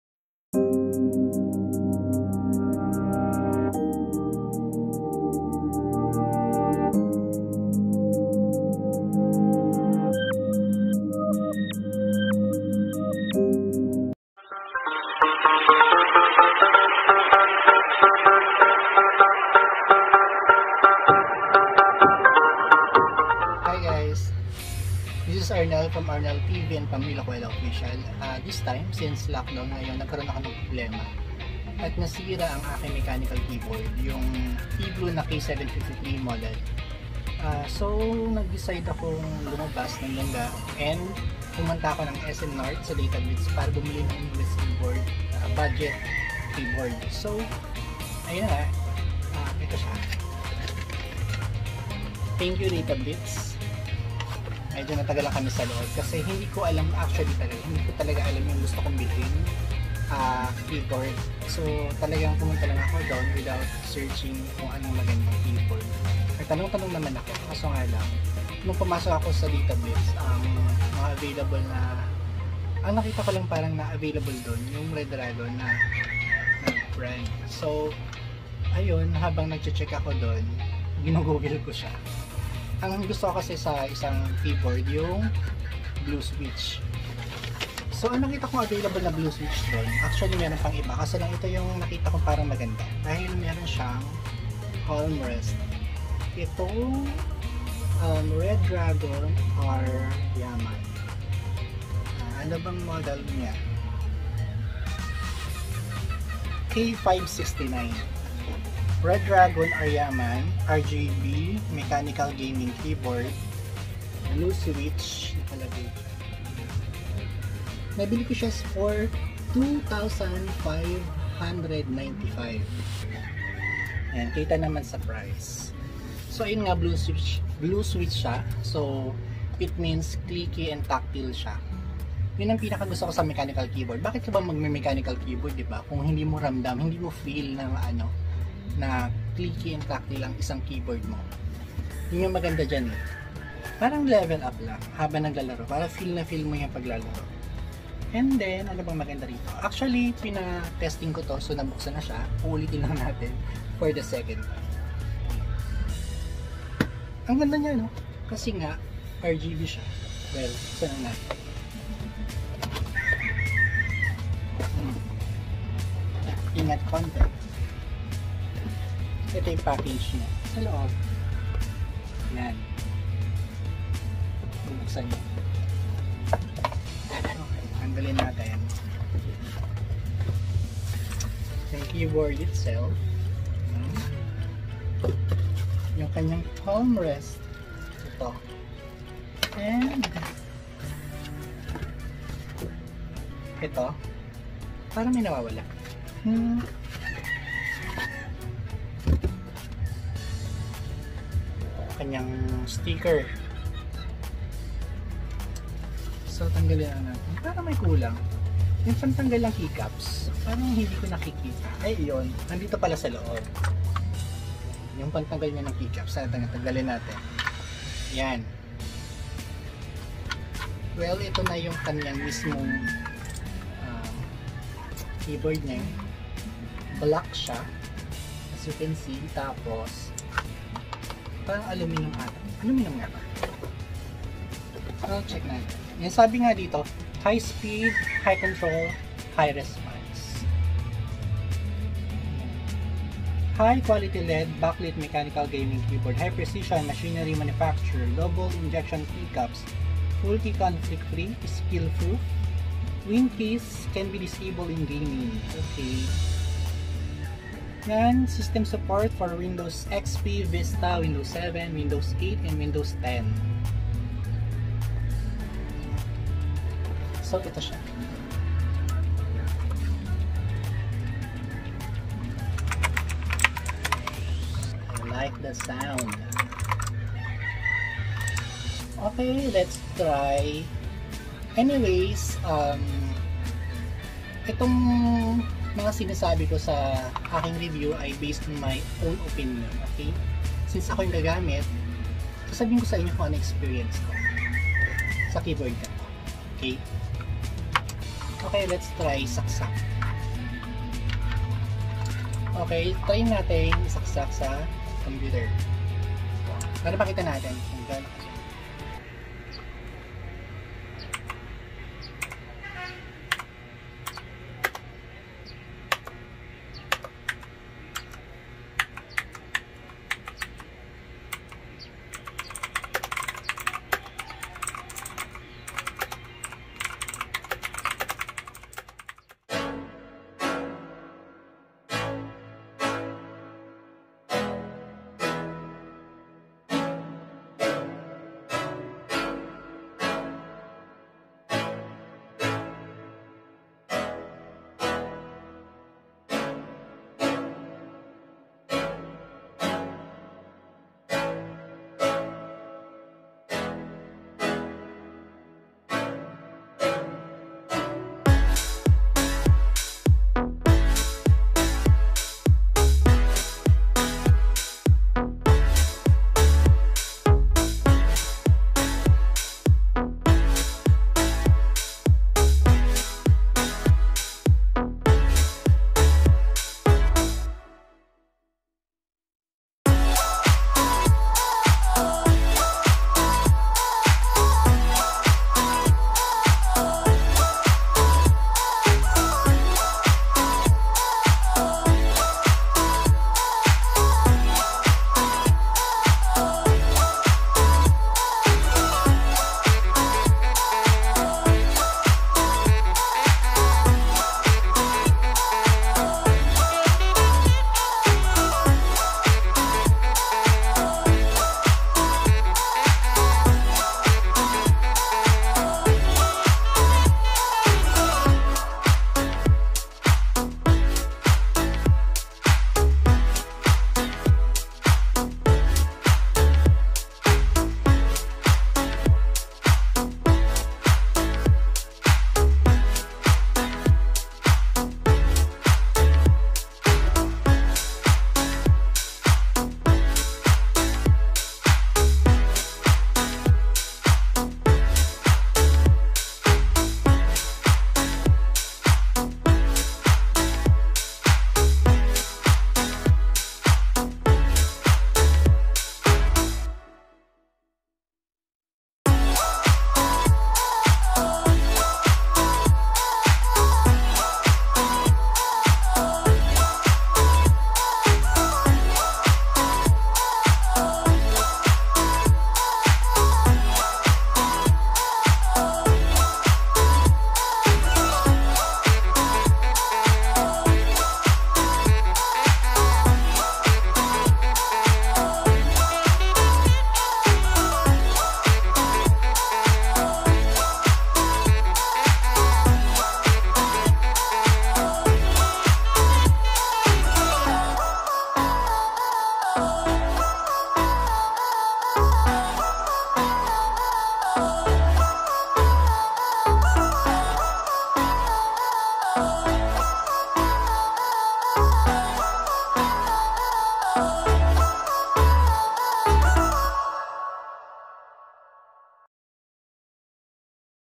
Budget Mechanical Keyboard 2020 Redragon Aryaman
Unboxing and Review of Budget Mechanical Keyboard 2020 Redragon Aryaman K569 RGB
Unboxing : 2:40
Typing Test : 5:07
Color Effects: 5:29